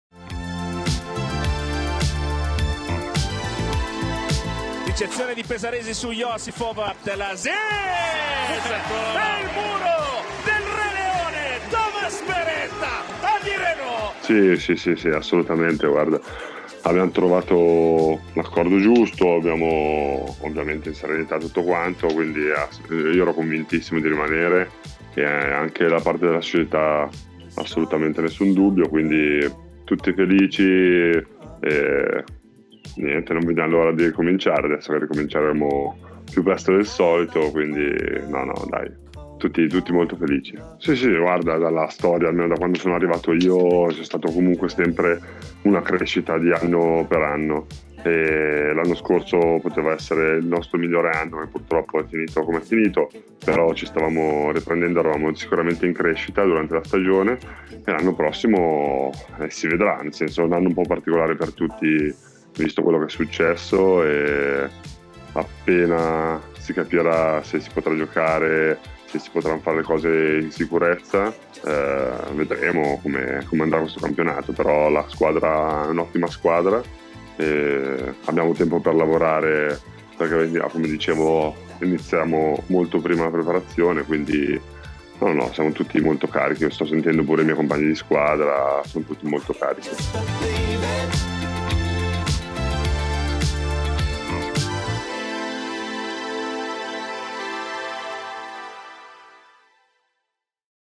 StrongerTOGETHER - Thomas Beretta rinnova con la Vero Volley Monza
Il centrale italiano giocherà un'altra stagione con la casacca della prima squadra maschile del Consorzio Vero Volley.